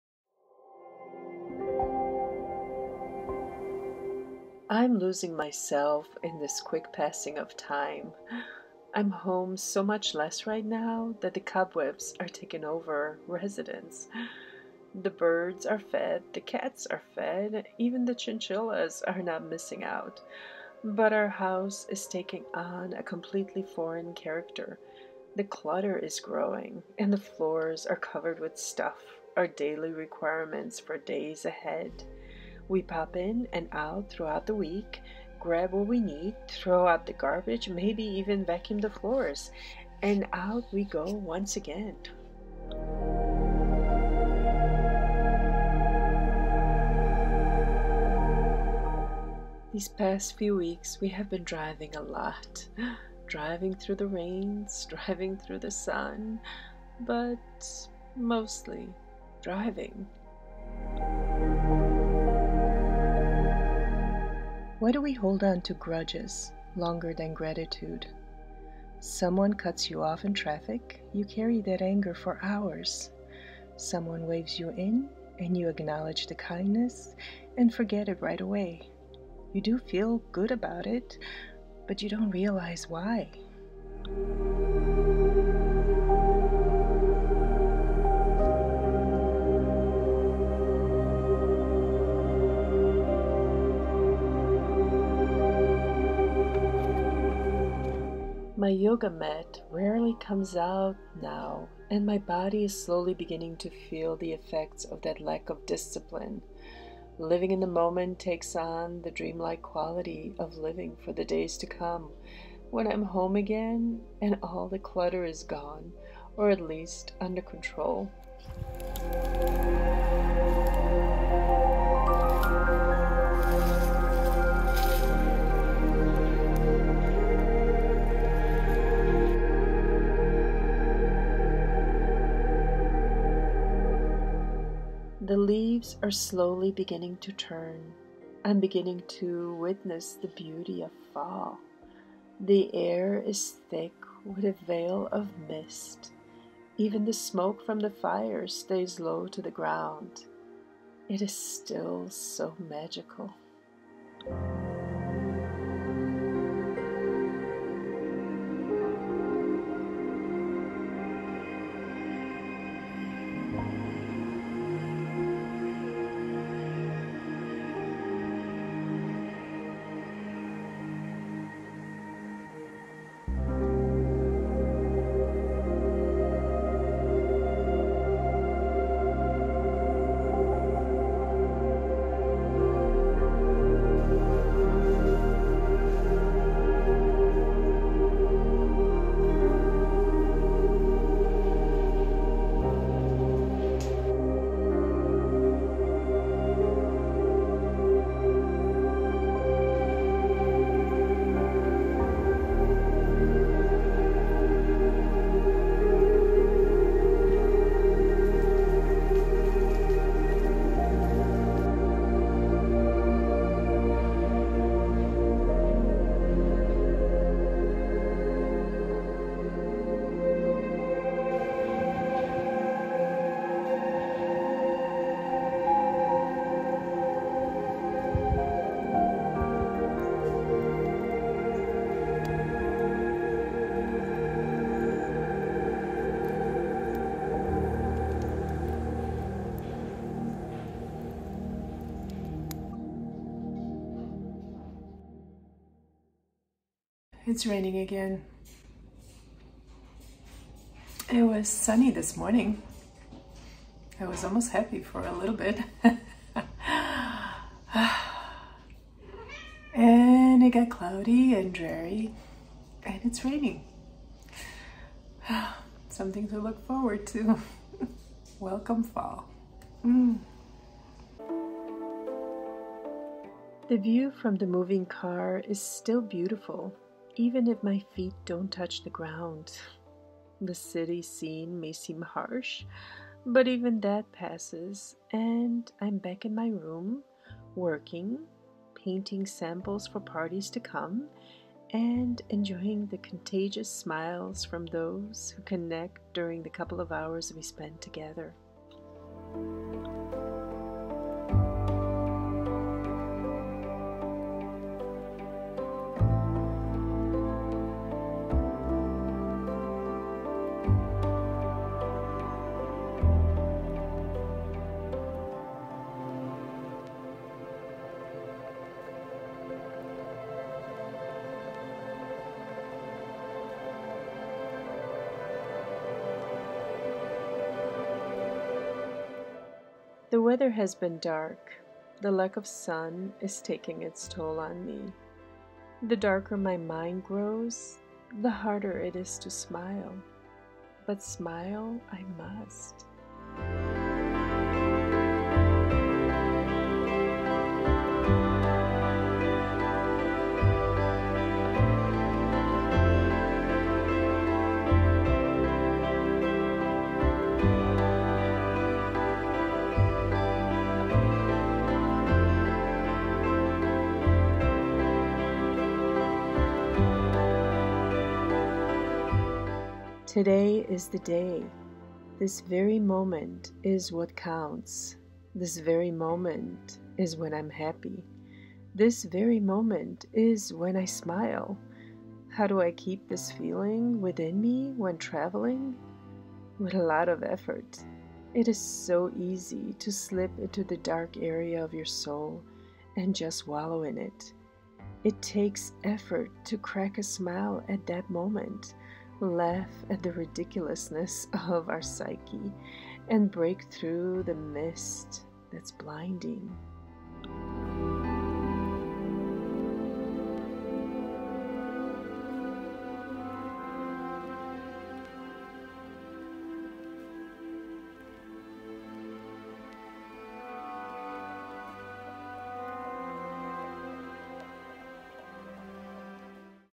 Smiling through the dark days of Fall
I feel like I'm repeating myself a lot these days.  The constant mantra of smile and be kind and find joy in everything around you, is what these videos are about.  The rest is the everyday of travel and work and very little play right now.  Digging myself out of the dark hole is more common during the month of October.  How long does it take me?  Sometimes few minutes, sometimes a full day.  It all depends on the day.
I wish I could show scenes of mountains and trees and a little bit of van life, but my life right now consists of cars and planes and one night stands in hotel rooms :)))
I do offer some views of a little bit of nature that I encounter.  Still no mountains or forests or cabins in the woods, but a tree is a tree, and that magic is real.